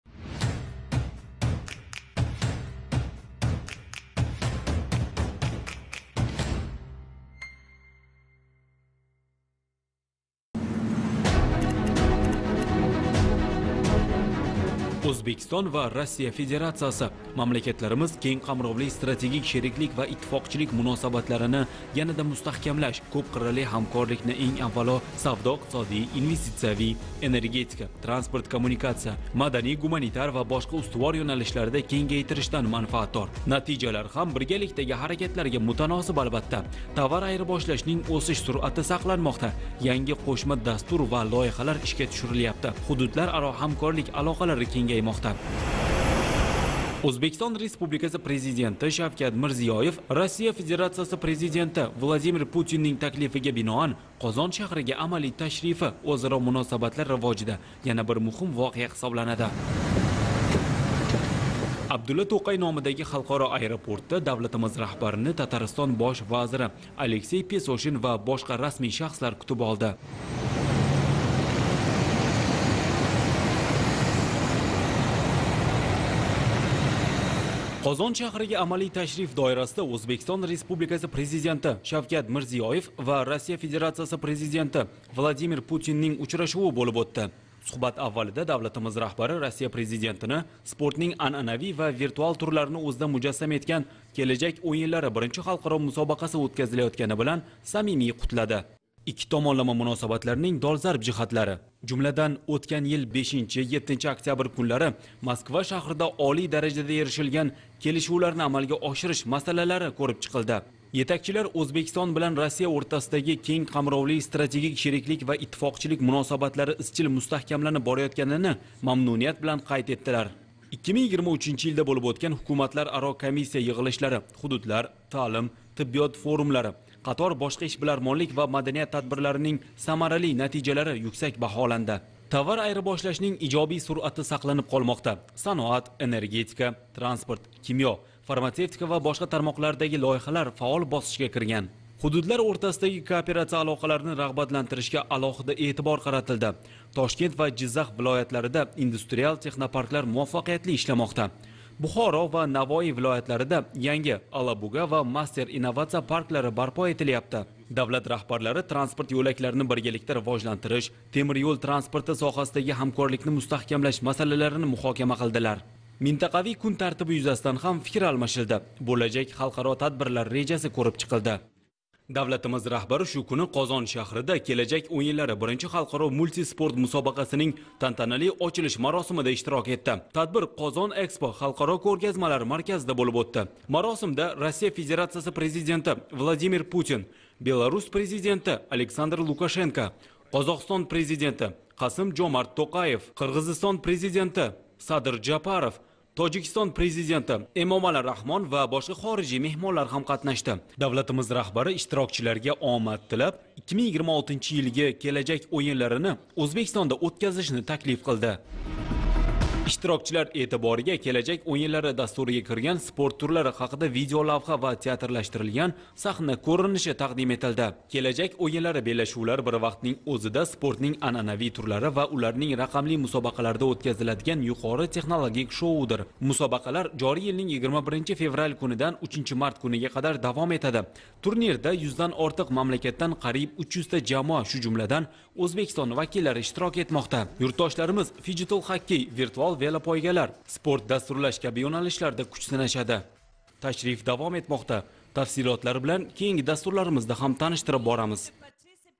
Шавкат Мирзиёев “Келажак ўйинлари” биринчи халқаро мусобақанинг очилишида иштирок этди
21 февраль куни Қозон шаҳрига амалий ташриф доирасида Ўзбекистон Республикаси Президенти Шавкат Мирзиёев ва Россия Федерацияси Президенти Владимир Путиннинг учрашуви бўлиб ўтди.

Суҳбат аввалида давлатимиз раҳбари Россия Президентини спортнинг анъанавий ва виртуал турларини ўзида мужассам этган “Келажак ўйинлари” биринчи халқаро мусобақаси ўтказилаётгани билан самимий қутлади.

Икки томонлама муносабатларнинг долзарб жиҳатлари, жумладан, ўтган йил 5-7 октябрь кунлари Москва шаҳрида олий даражада эришилган келишувларни амалга ошириш масалалари кўриб чиқилди.

Етакчилар Ўзбекистон билан Россия ўртасидаги кенг қамровли стратегик шериклик ва иттифоқчилик муносабатлари изчил мустаҳкамланиб бораётганини мамнуният билан қайд этдилар.

2023 йилда бўлиб ўтган Ҳукуматлараро комиссия йиғилишлари, ҳудудлар, таълим, тиббиёт форумлари, қатор бошқа ишбилармонлик ва маданият тадбирларининг самарали натижалари юксак баҳоланди.

Товар айирбошлашнинг ижобий суръати сақланиб қолмоқда. Саноат, энергетика, транспорт, кимё, фармацевтика ва бошқа тармоқлардаги лойиҳалар фаол босқичга кирган.

Ҳудудлар ўртасидаги кооперация алоқаларини рағбатлантиришга алоҳида эътибор қаратилди.

Тошкент ва Жиззах вилоятларида индустриал технопарклар муваффақиятли ишламоқда. Бухоро ва Навоий вилоятларида янги “Алабуга” ва “Мастер” инновация парклари барпо этилмоқда.

Давлат раҳбарлари транспорт йўлакларини биргаликда ривожлантириш, темир йўл транспорти соҳасидаги ҳамкорликни мустаҳкамлаш масалаларини муҳокама қилдилар.

Минтақавий кун тартиби юзасидан ҳам фикр алмашилди, бўлажак халқаро тадбирлар режаси кўриб чиқилди.